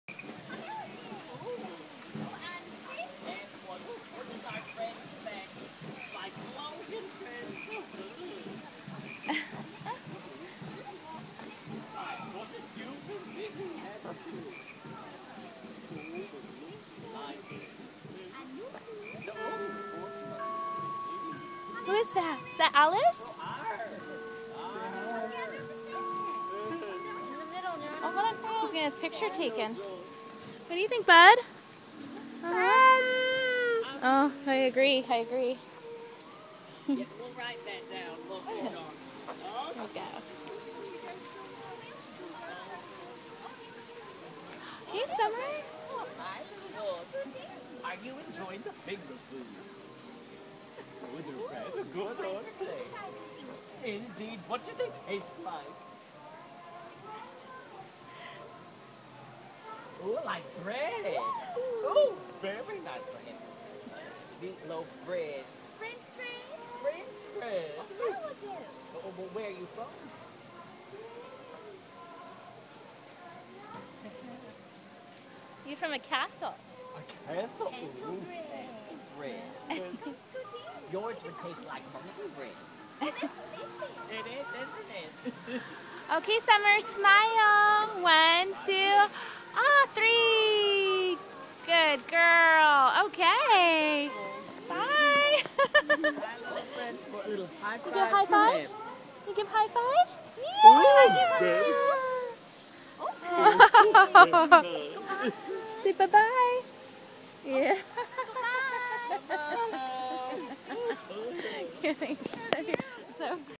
Summer meets the Mad Hatter
Talkn about bread- summer age almost three